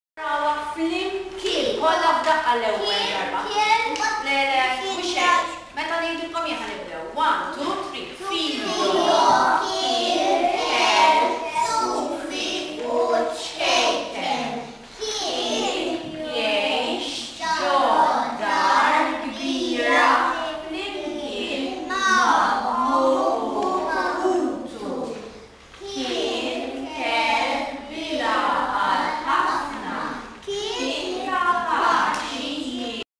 group reading.MOV
L-istudenti jaqraw bicciet mill-istorja min fuq handout bhala grupp.